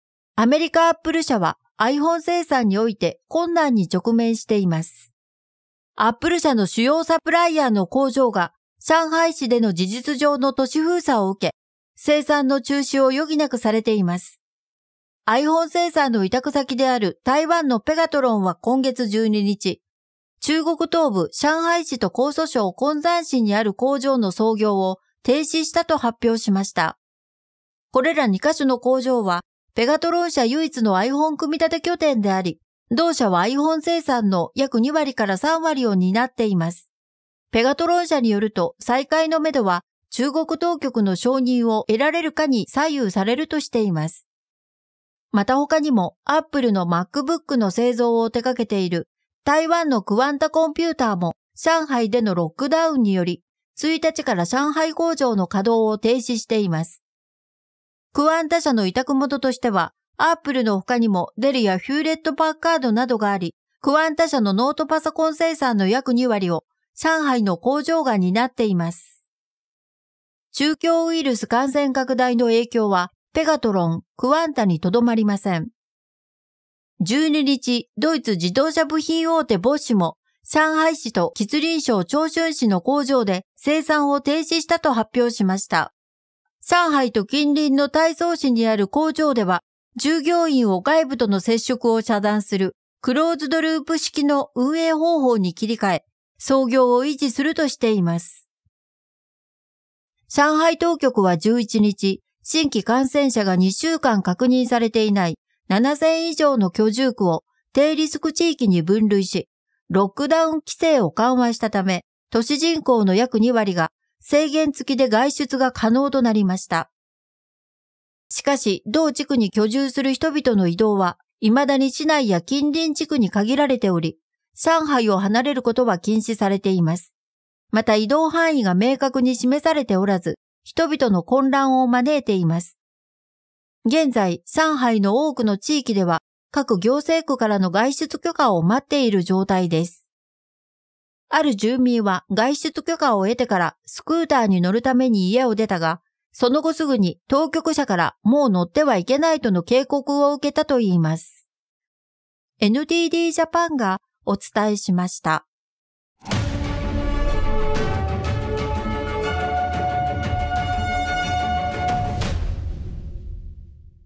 Apple委託先の中国工場 相次ぎ操業停止 ロックダウン受け
米Apple社は「iPhone」生産において困難に直面しています。
Apple社の主要サプライヤーの工場が、上海市での事実上の都市封鎖（ロックダウン）を受け、生産の中止を余儀なくされています。

預金種目：普通預金
口座番号：2158157
口座名義：ﾄｸﾋ)ｴﾎﾟｯｸﾒﾃﾞｨｱ